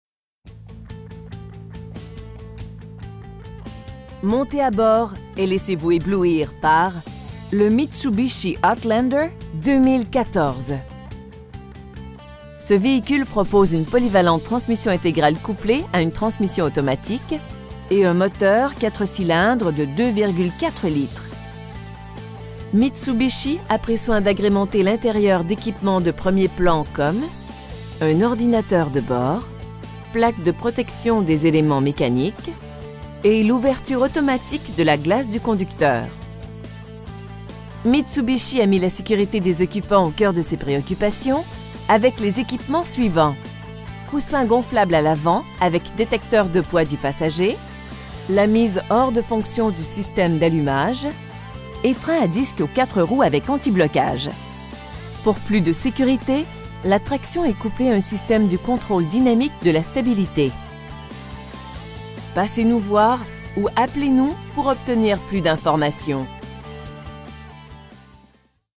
2014 Mitsubishi Outlander in Charlemagne Qc, QC J5Z 0B5
Montez �� bord et laissez-vous ��blouir par le Mitsubishi Outlander 2014. Ce v��hicule propose une polyvalente transmission int��grale coupl��e �� une transmission automatique et un moteur 4 cylindres de 2,4 L. Mitsubishi a pris soin d'agr��menter l'int��rieur d'��quipement de premier plan, comme: un ordinateur de bord, une banquette arri��re �� dossiers rabattables s��par��s, climatiseur avec commande automatique de temp��rature, plaques de protection des ��l��ments m��caniques, galerie de toit, essuie-glace arri��re, et l'ouverture automatique de la glace du conducteur. Vous et vos passagers appr��cierez la sonorit�� de la cha��ne st��r��o, qui comprend une lecteur CD et une connexion MP3, commandes audio sur le volant, et 6 haut-parleurs parfaitement positionn��s. Mitsubishi a mis la s��curit�� des occupants au coeur de ses pr��occupations avec les ��quipements suivants: coussins gonflables �� l'avant avec d��tecteur de poids du passager, coussins gonflables lat��raux �� l'avant, syst��me antipatinage, syst��me d'assistance au freinage, la mise hors de fonction du syst��me d'allumage, et freins �� disque aux 4 roues avec antiblocage. Pour plus de s��curit��, l'entra��nement est coupl�� �� un syst��me du contr��le dynamique de la stabilit��. Nous offrons la plus agr��able exp��rience d'achat, sans oublier les prix les plus concurrentiels. Passez nous voir ou appelez-nous pour obtenir plus d'information.